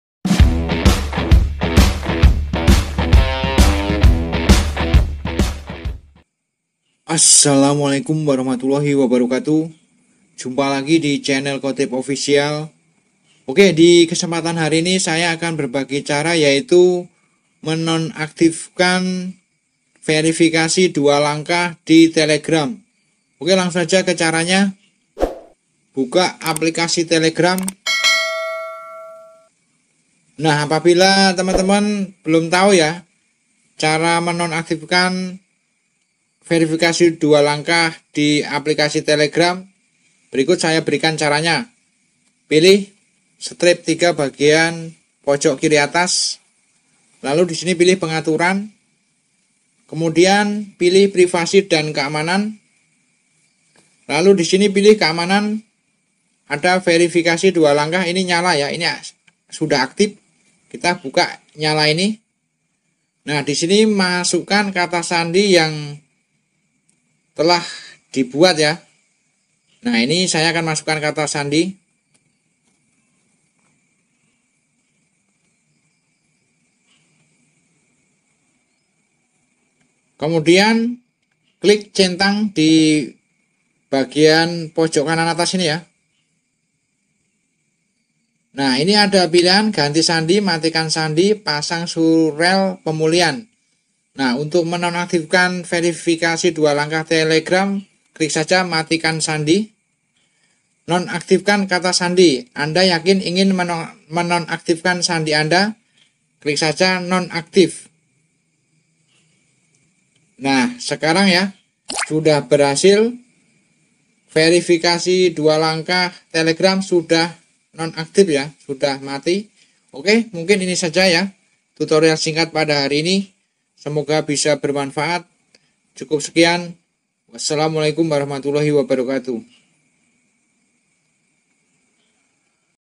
Cara Menonaktifkan Verifikasi Dua Langkah Telegram
Kita perlu menambah keamanan akun telegram dengan cara mengaktifkan verifikasi dua langkah,nah di video ini saya berbagi tutorial cara menonaktifkan verifikasi dua langkah telegram dan semoga bermanfaat verifikasi2langkahtelegram verifikasidualangkah#